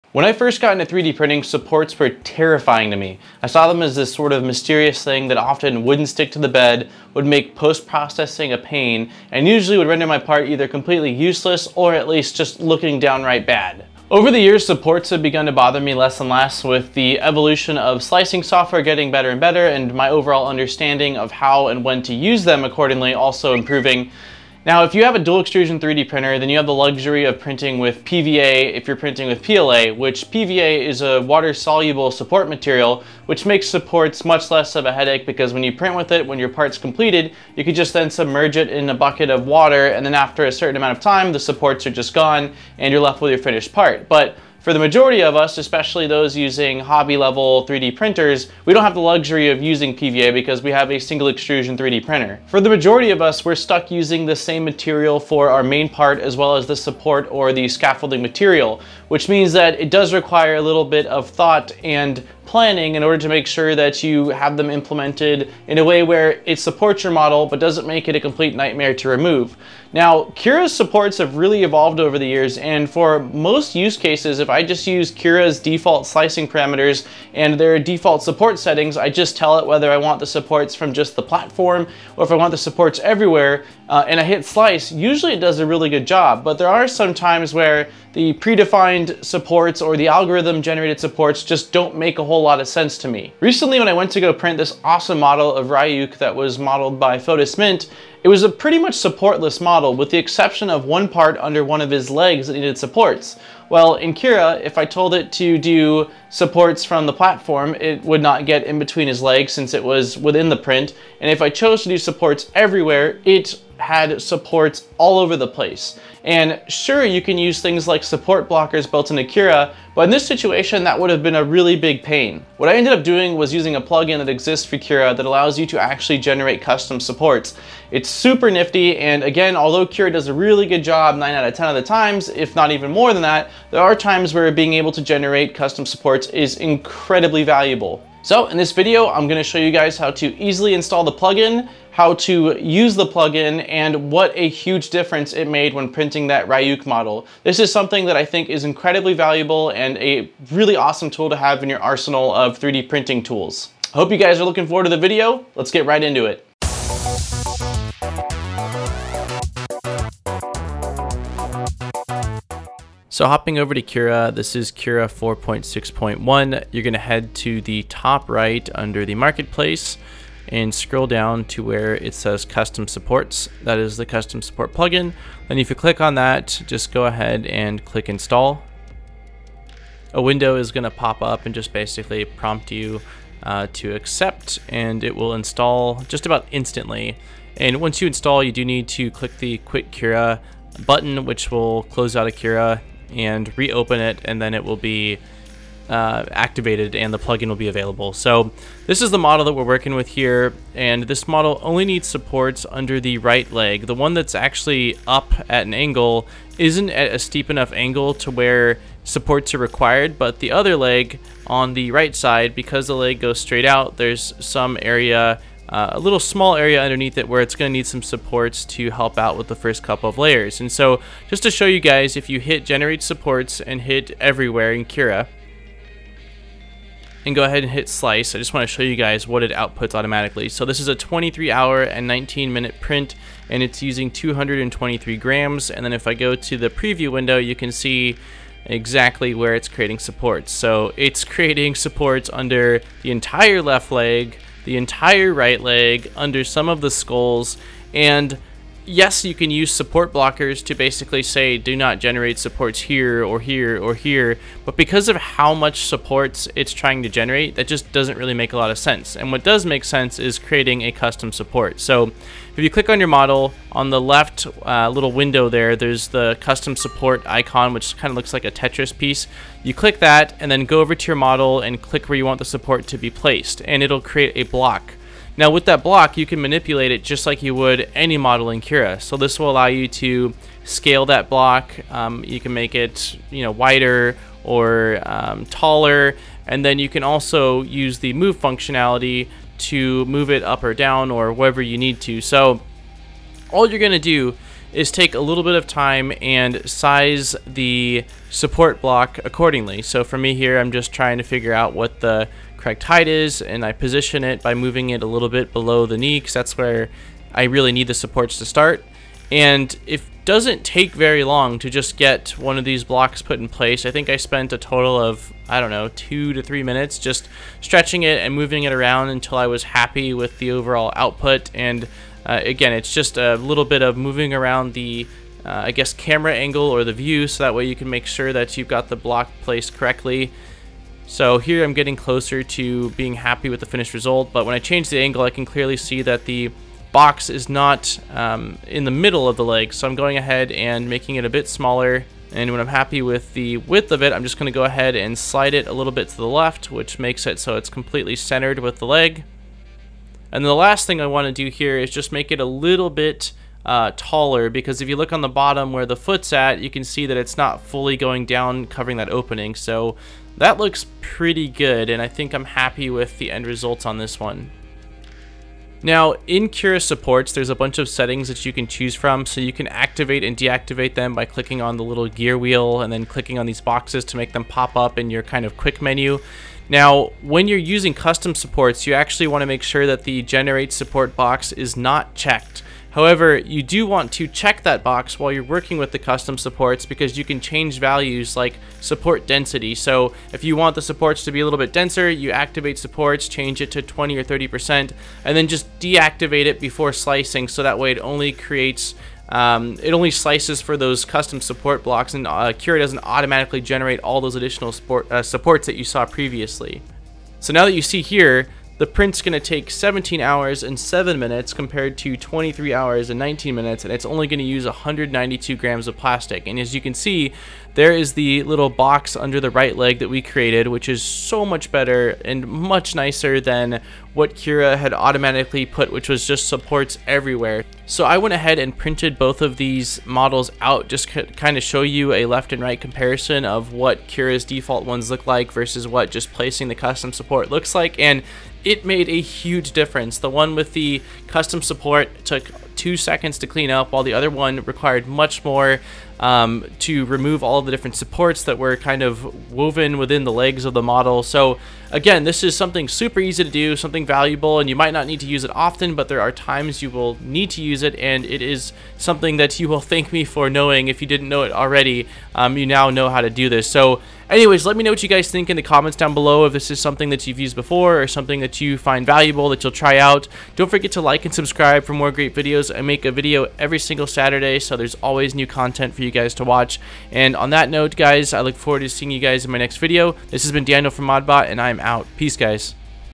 Easy Custom Manual Supports In Cura (4.6.1) For 3d Printing
In this video we take a look at how I use a custom support plugin in Cura to allow me to place supports only where I need them. This is such an awesome plugin that is very handy for times when Cura is not able to create supports exactly how you want them. After you watch this video you will be off and creating custom supports of your own!